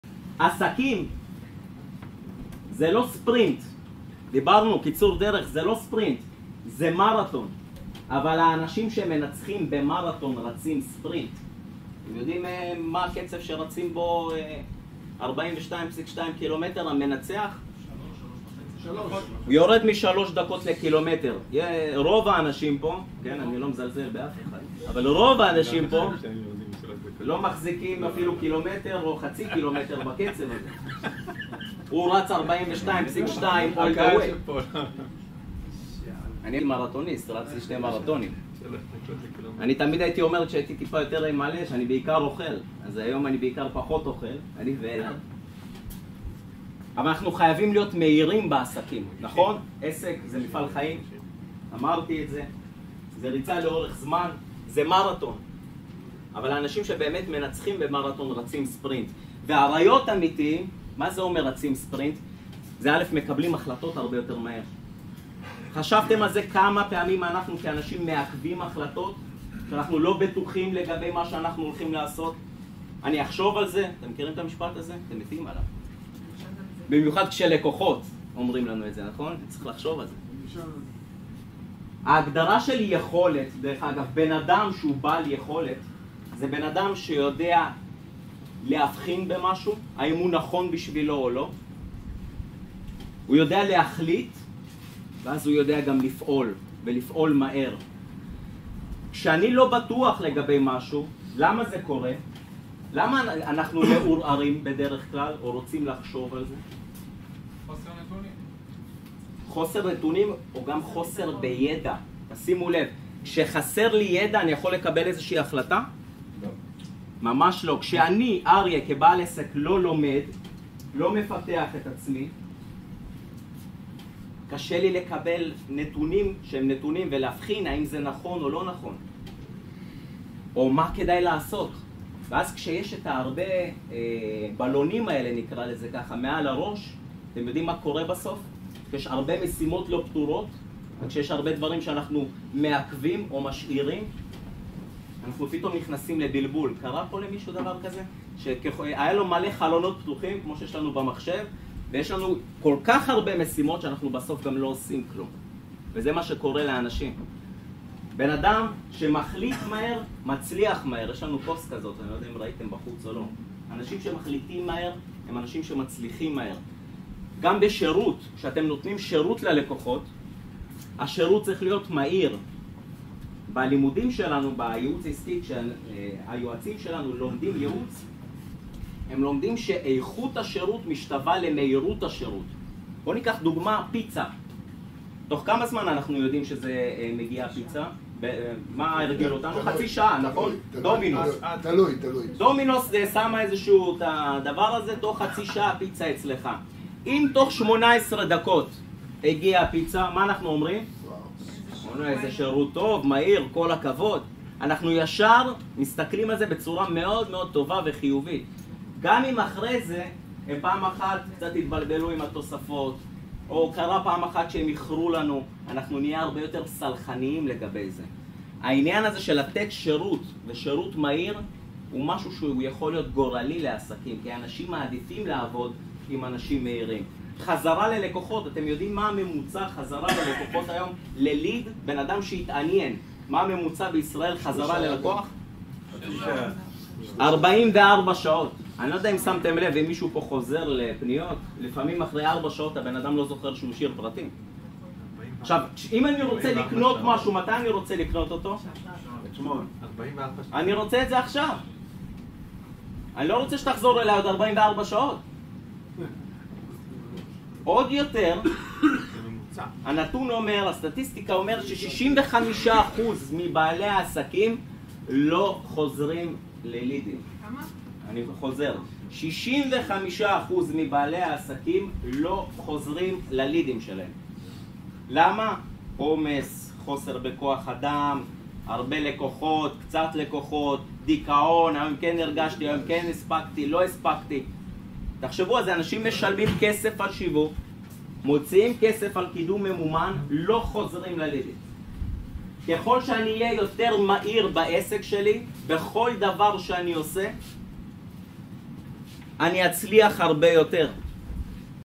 לרוץ מרתון ומהר
עסקים זה לא ספרינט, זה מרתון,
אבל האנשים שמנצחים מרתון רצים ספרינט.